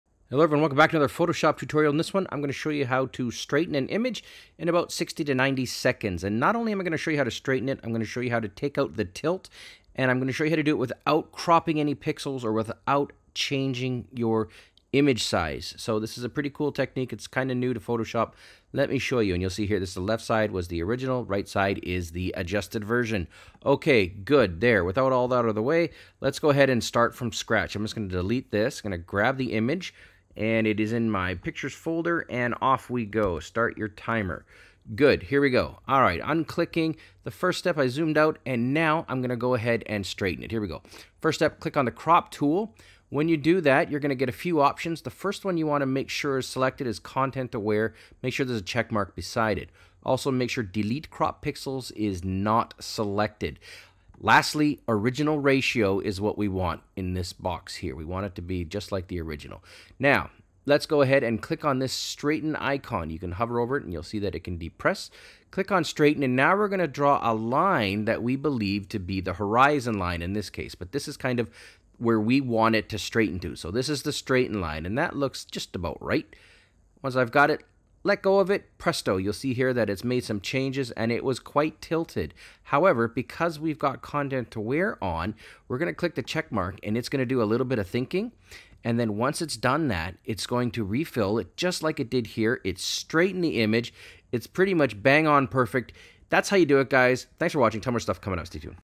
How To Straighten An Image (Remove Tilt) In 90 Seconds (NO CROPPING) - Photoshop CC
In this quick tutorial I show you step by step how to straighten an image and remove any tilt.  Select the crop tool and set the ratio to original, make sure content aware has a checkmark beside it and delete cropped pixels is not selected.  Click on straighten and draw a line that corresponds to the intended horizon line.